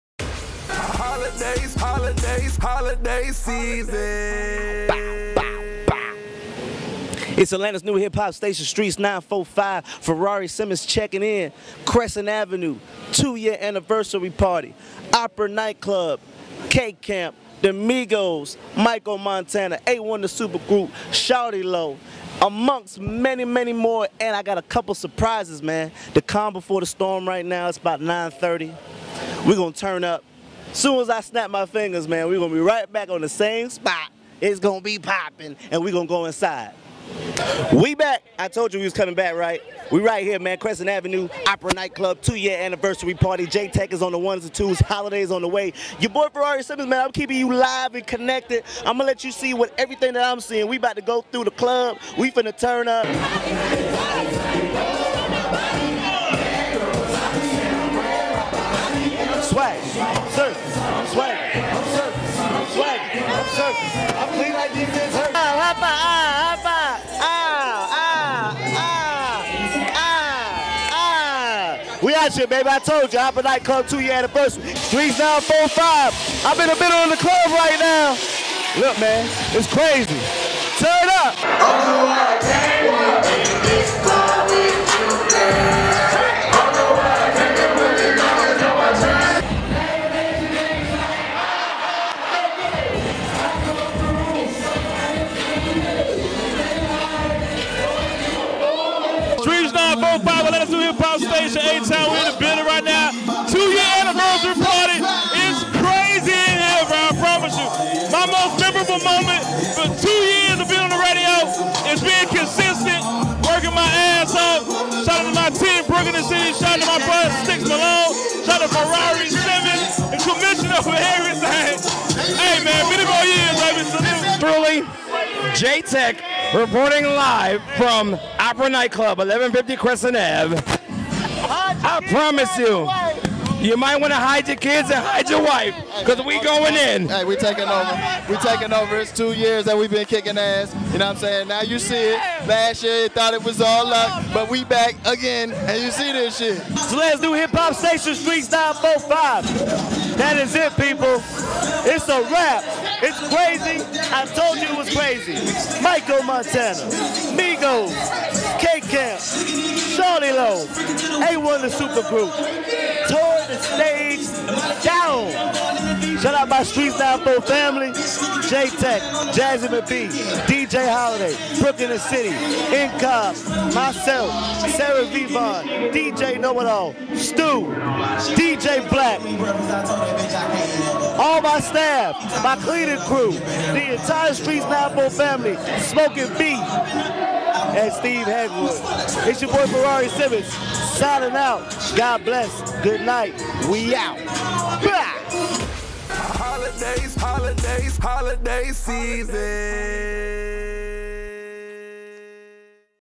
Streetz 94.5 2nd Yr Anniversary Streetz 94.5 Live Recap With Ferrari Simmons
Streetz 94.5 celebrates 2 years of service to the city of Atlanta at Club Opera.

Atlantas New Hip-Hop Station was joined by The Migos, K Camp, Travis Porter,  Myko Montana, Shawty Lo & A1 the Super Group to commemorate the event at Opera Night Club.